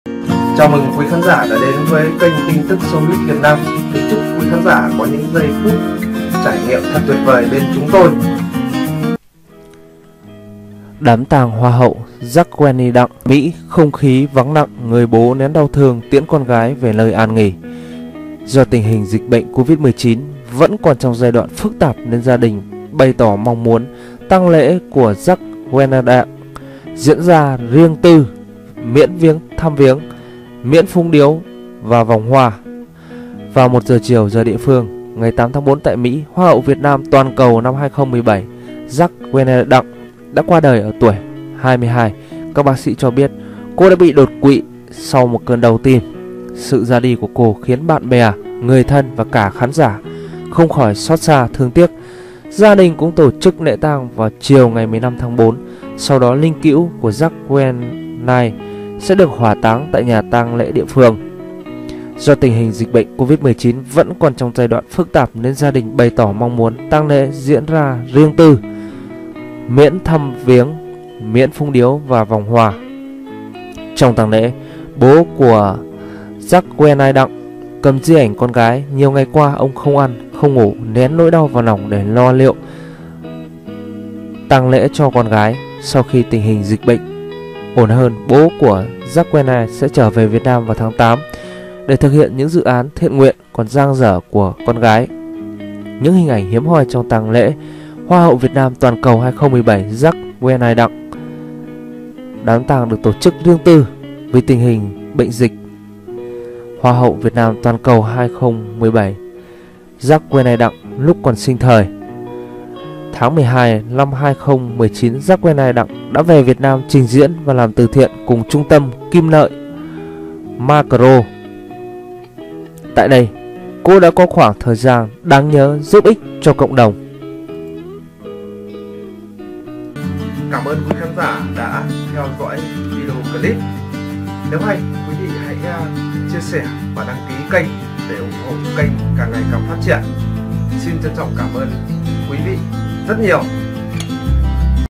ĐÁM TANG Hoa hậu Jacqueline Đặng tại Mỹ: KHÔNG KHÍ VẮNG LẶNG người bố nén đau thương TIỄN CON GÁI
Do tình hình dịch bệnh Covid-19 vẫn còn trong giai đoạn phức tạp nên gia đình bày tỏ mong muốn tang lễ của Jacqueline Đặng diễn ra riêng tư, miễn thăm viếng, miễn phúng điếu và vòng hoa.